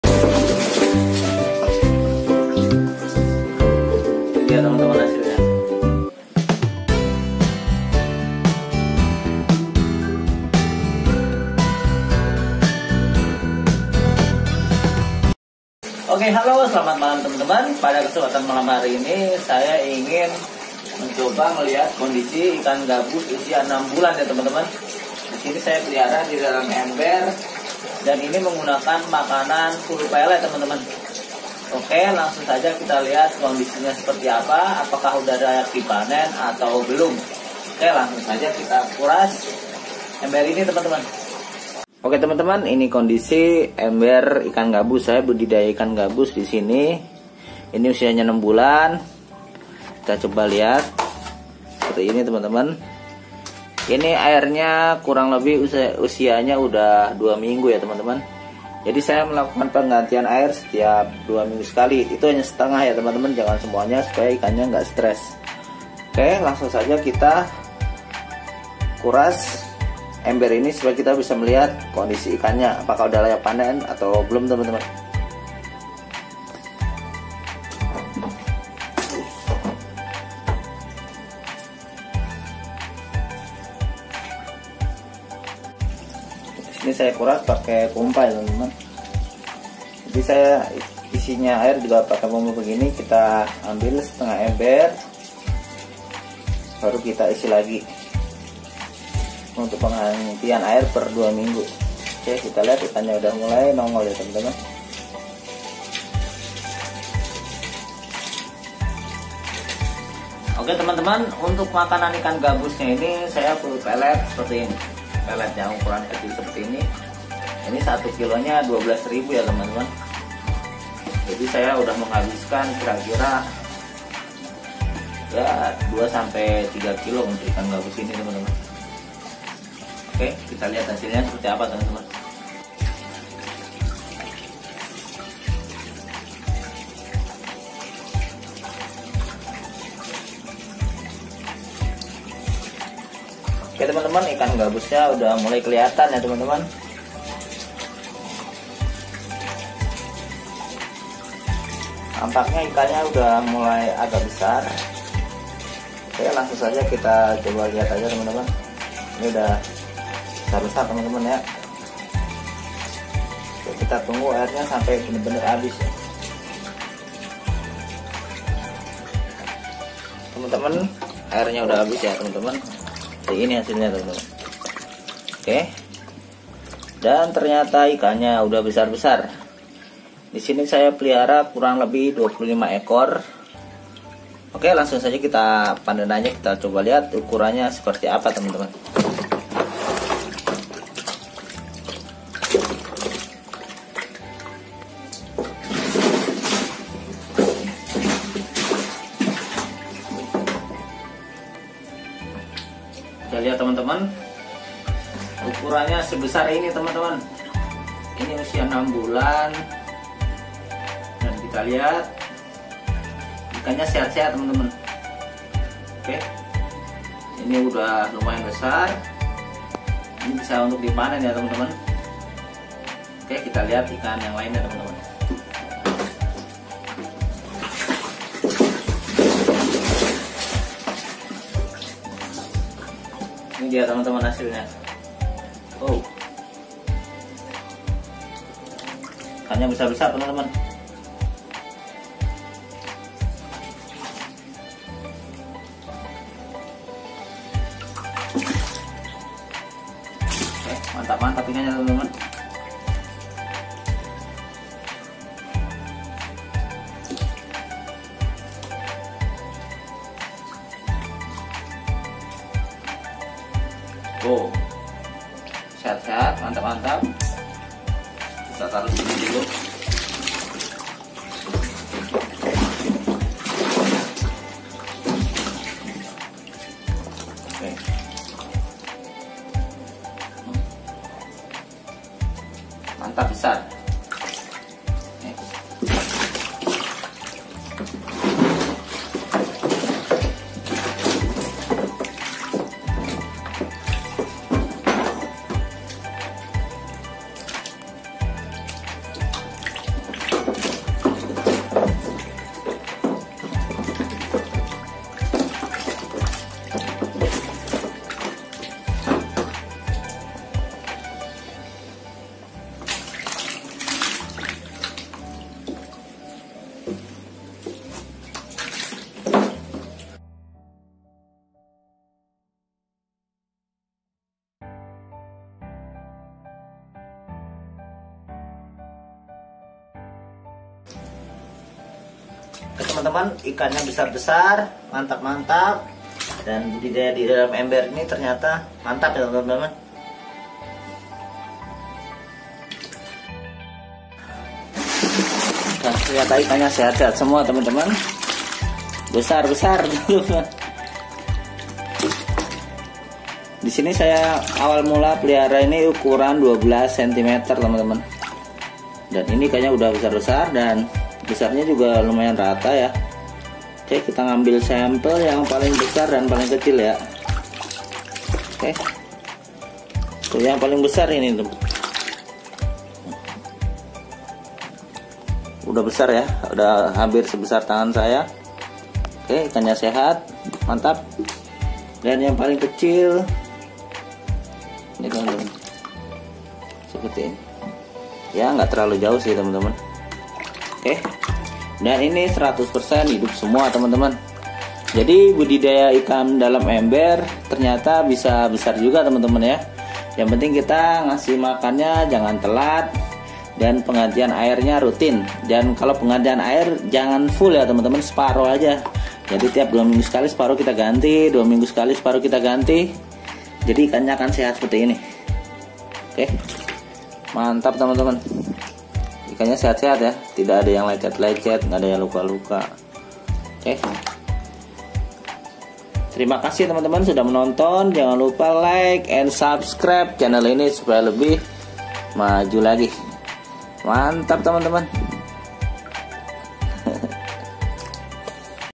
TERNYATA BISA !!!! Budidaya ikan gabus di dalam ember full pelet 6 bulan panen
pada kesempatan kali ini saya ingin memanen ikan gabus saya yang berusia 6 bulan